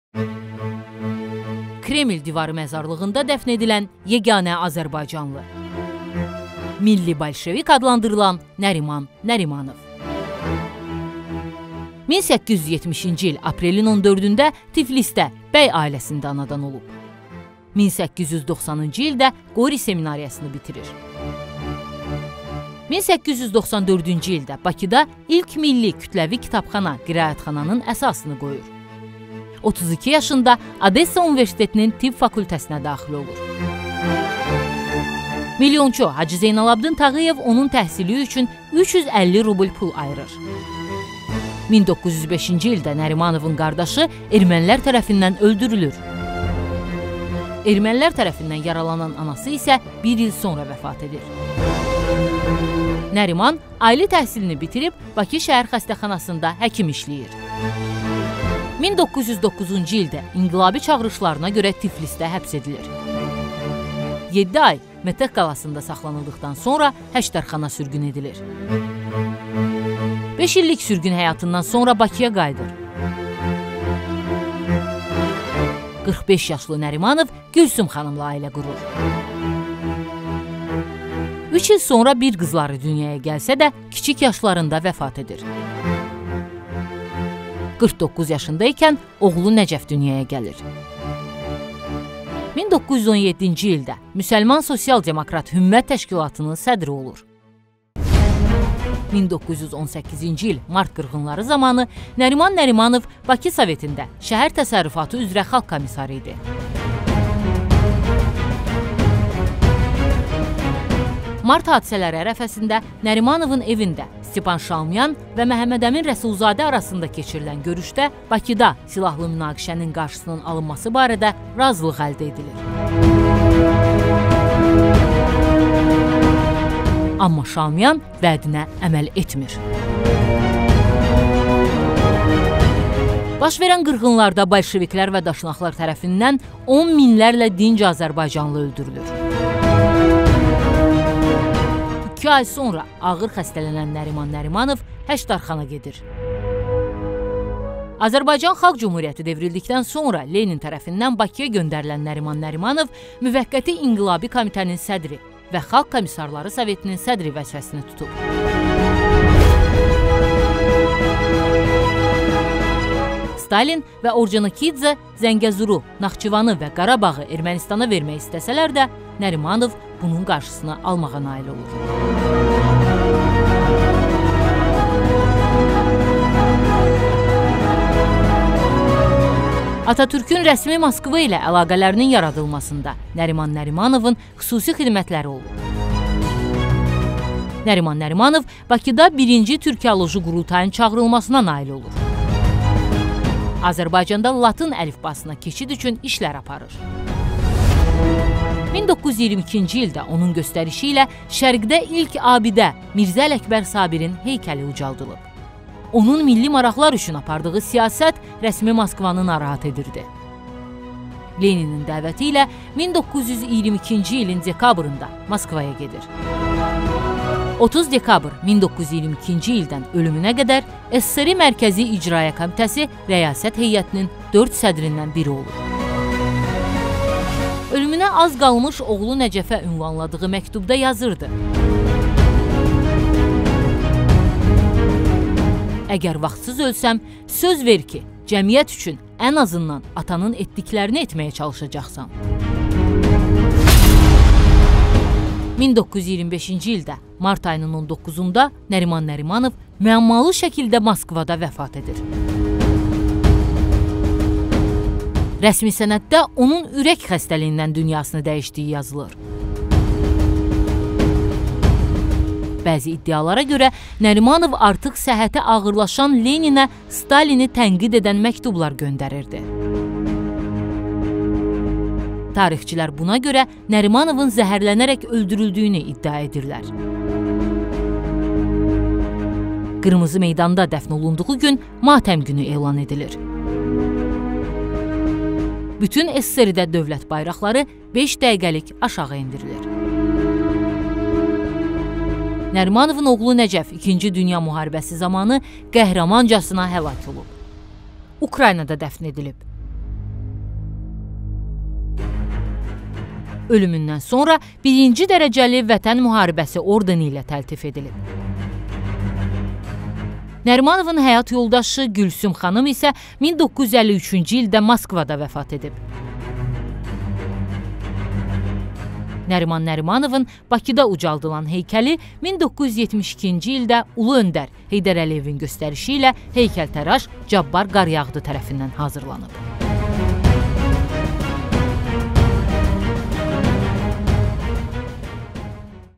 Nəriman Nərimanov kim idi?
Azərbaycan tarixinin ən çox müzakirə olunan şəxsiyyətlərindən biri,
müəmmalı şəkildə vəfat edən,
Moskvada Kremlin divarları yaxınlığında dəfn edilən Nəriman Nərimanov...